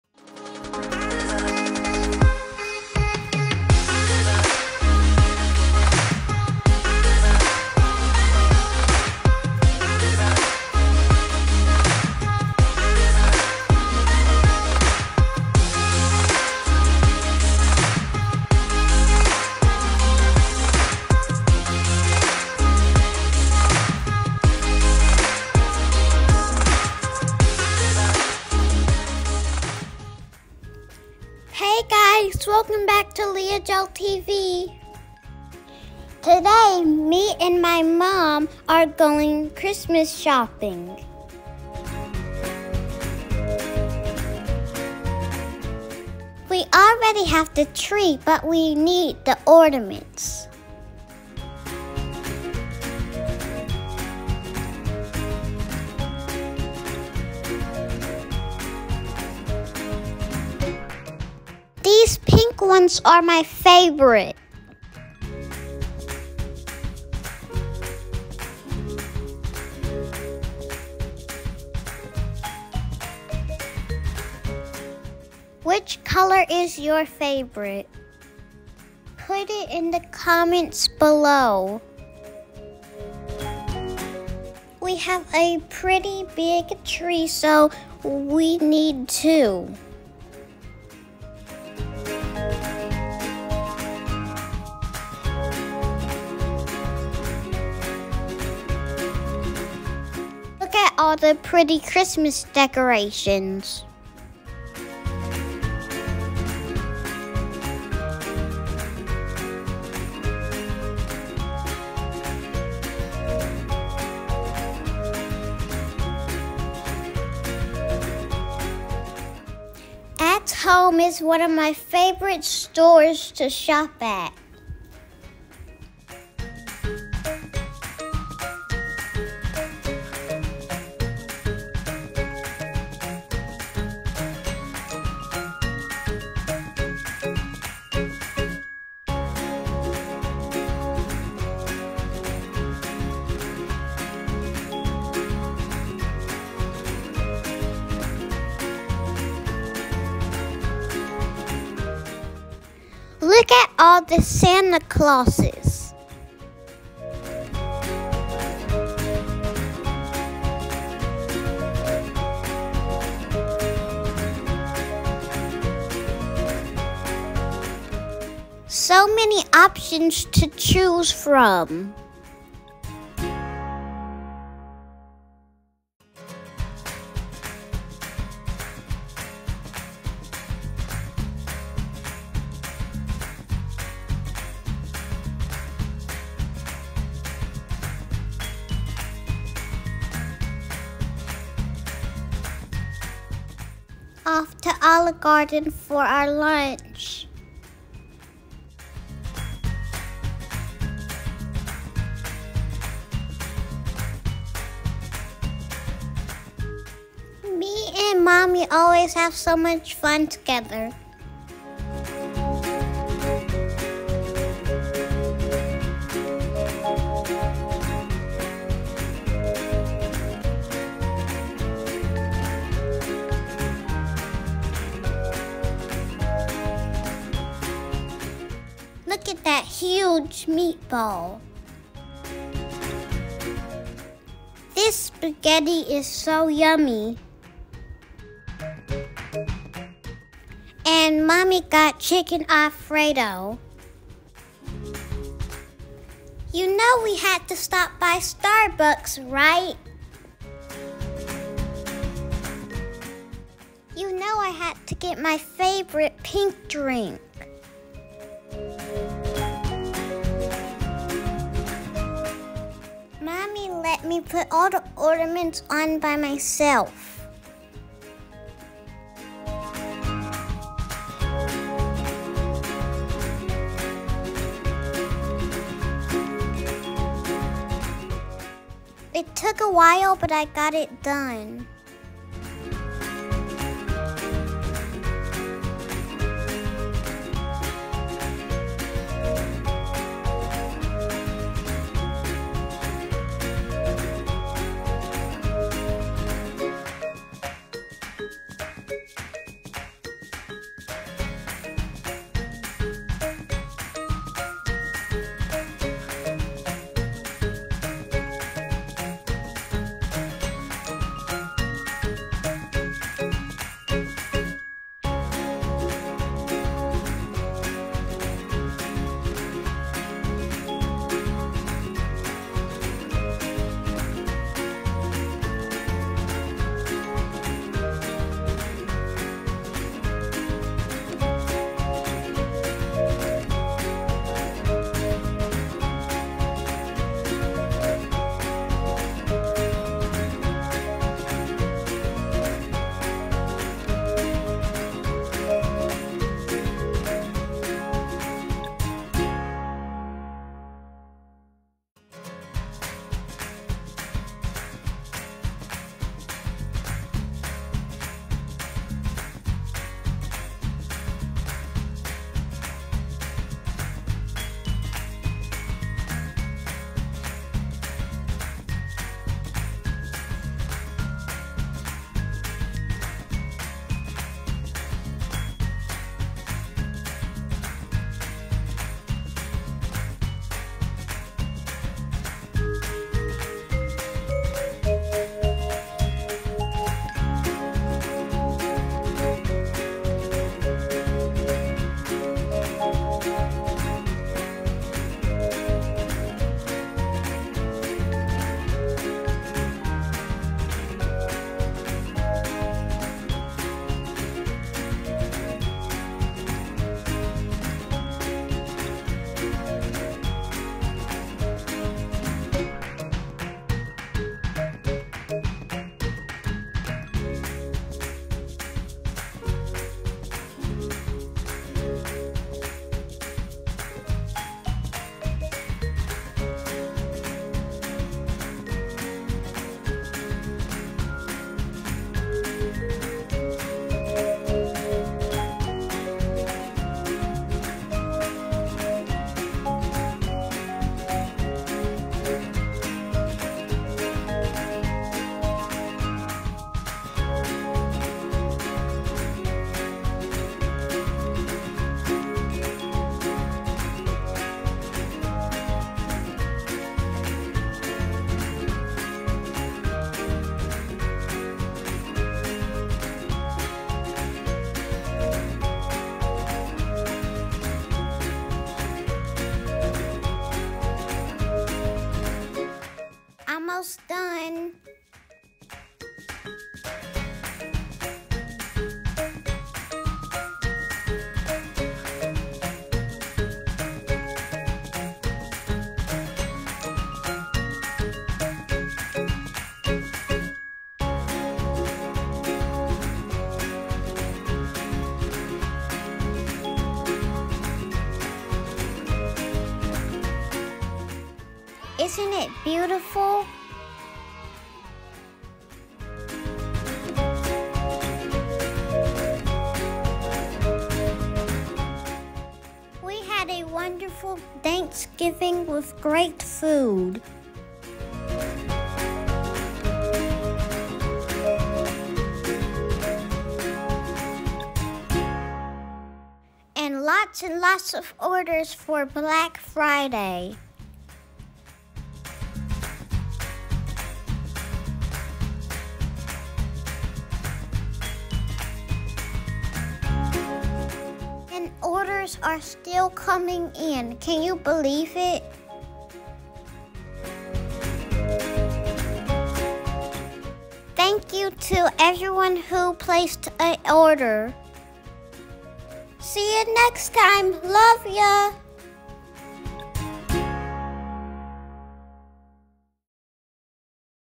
Holiday 2021 Mommy and Me Vlog
WE WENT CHRISTMAS SHOPPING AT AT HOME, LUNCH AT OLIVE GARDEN, AND STARBUCKS. THANKSGIVING WAS SO YUMMY! MY BLACK FRIDAY SALE WAS A SUCCESS! THANK YOU TO EVERYONE WHO PURCHASED FROM MY BOUTIQUE!

FACEBOOK: LEIGHA JO APPAREL STUDIO